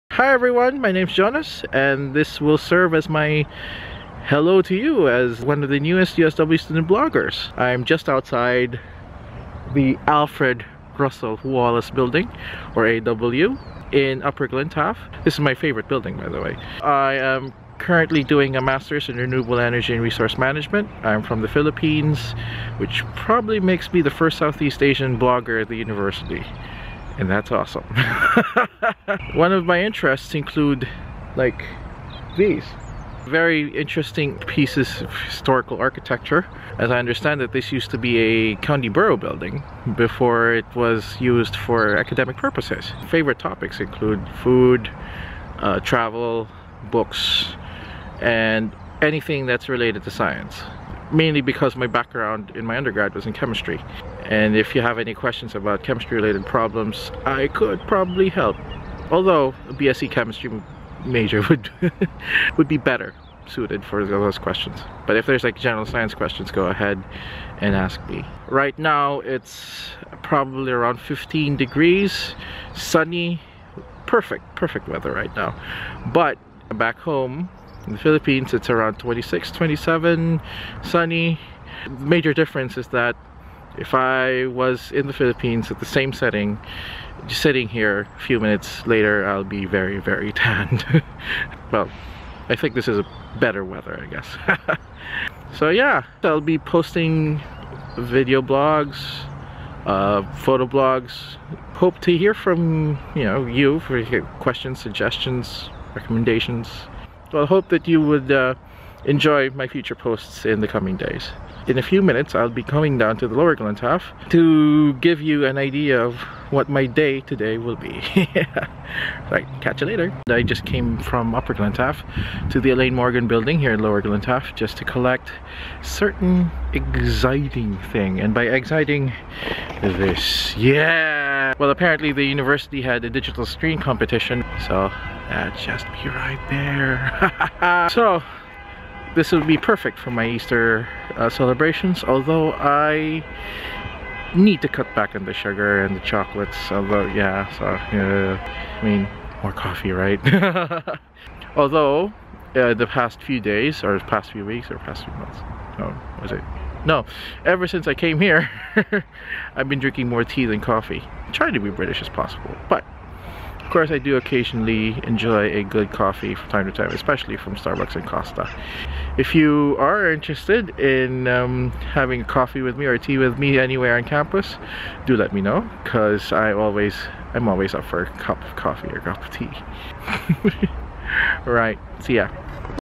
MEET JONAS DUMDUM! | Southeast Asia Student Vlogger
Meet Jonas, our newest Student Vlogger! He's from the Philippines and studies MSc Renewable Energy and Resource Management Student!